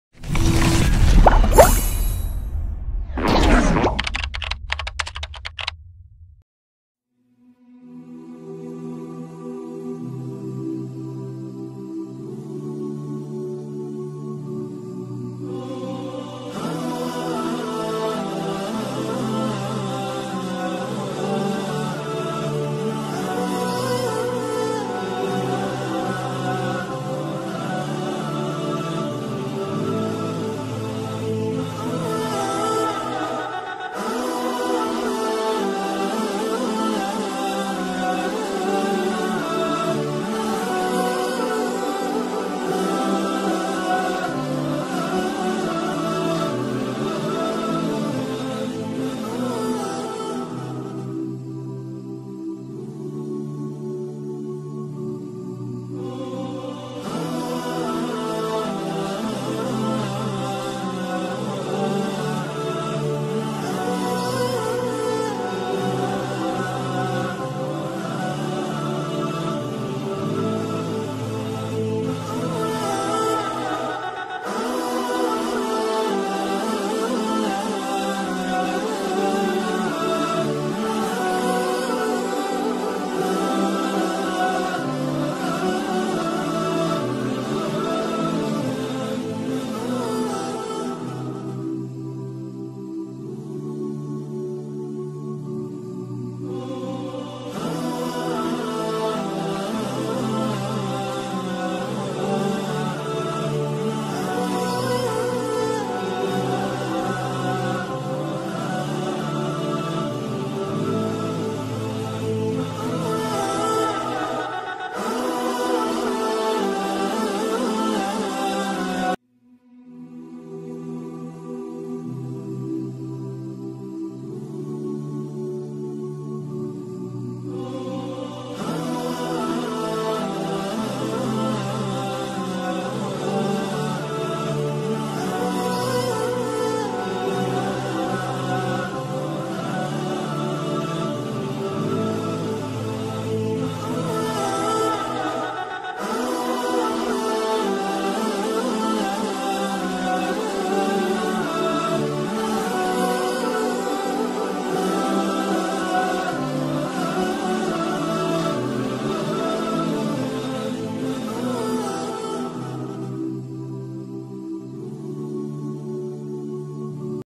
SMP IT YABIS - Aktivitas Pagi "Murojaah Hafalan Al-Qur’an"
Setiap harinya siswa -siswi serta dewan guru SMP YABIS BONTANG sebelum memasuki kelas dan melakukan aktivitas belajar mengajar, selalu mengawalinya dengan kegiatan Sholat Dhuha kemudian dilanjutkan Murojaah Hafalan Al-Qur’an untuk seluruh anak-anak didik kami dan dewan guru.